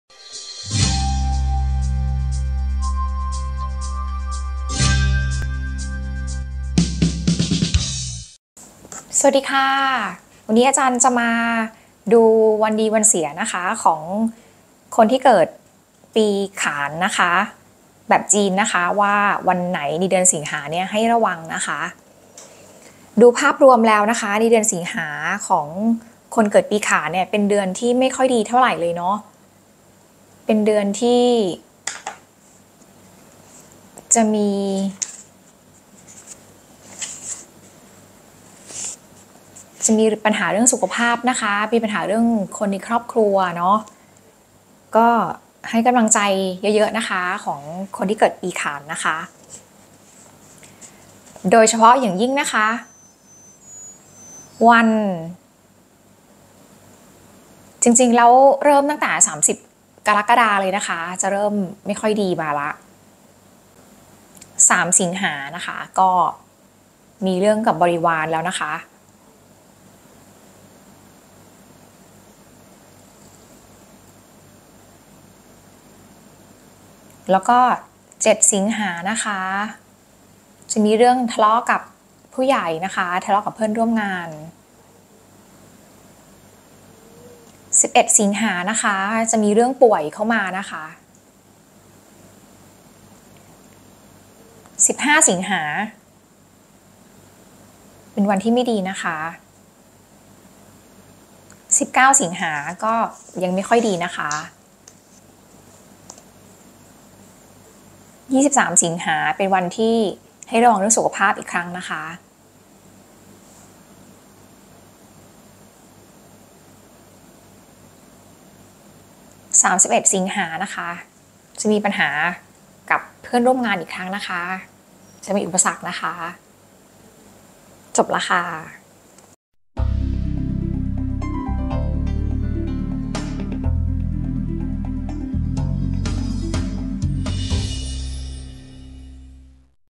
วันเสีย ของคนเกิดปีขาล เดือนสิงหาคม 2565 โดย อ.เจ้า Horo Master
จะมีวันไหนที่เป็นวันไม่ดี จะมีปัญหา มีความหงุดหงิดใจ ระวังเจ็บป่วย ไม่สบาย วันเสียเงิน ติดตามได้ในคลิปนี้
ดูดวงส่วนตัวกับอ.เจ้า Line@ : @847nshmn

PRvariety ข่าวประชาสัมพันธ์เพื่อสังคม